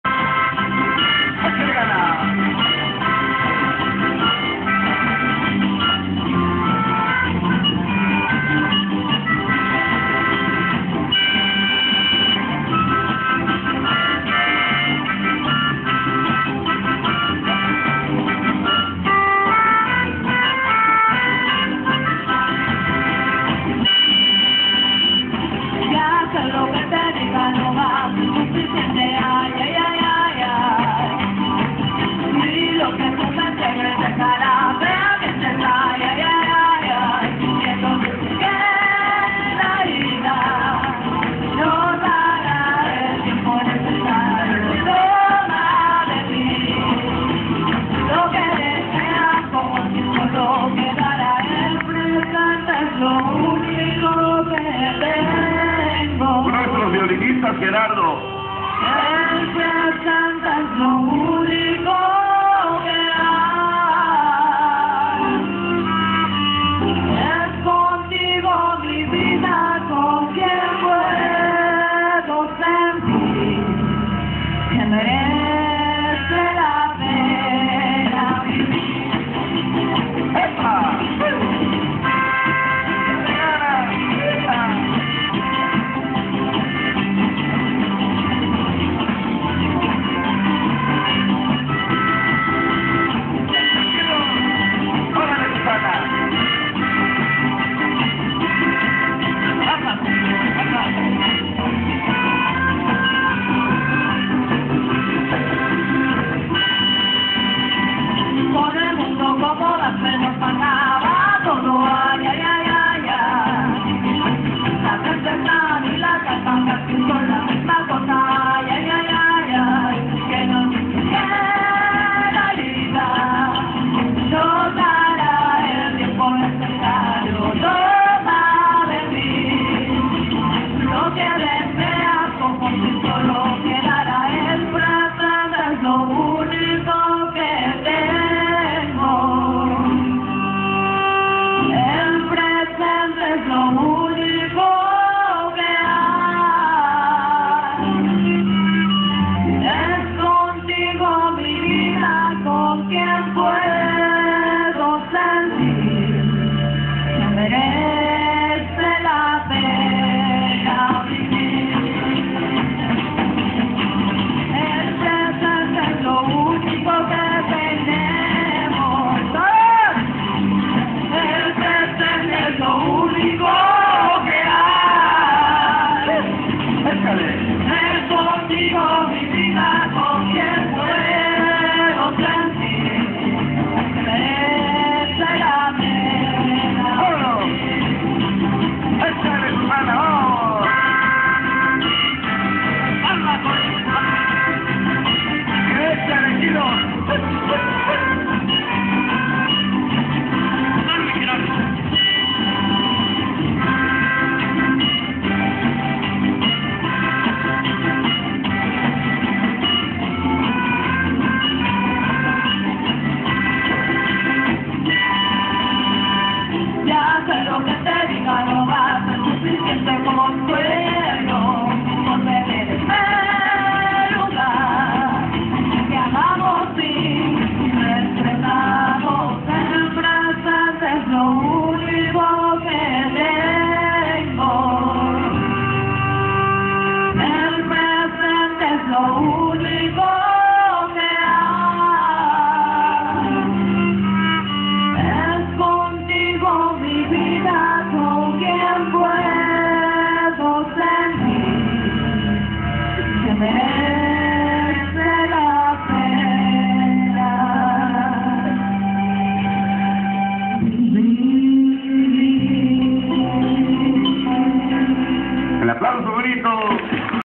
Rondalla del Cetis61
Esta es la segunda cancion que el profesor memo
y su ya reconocido grupo de musica cantaron
en el convivio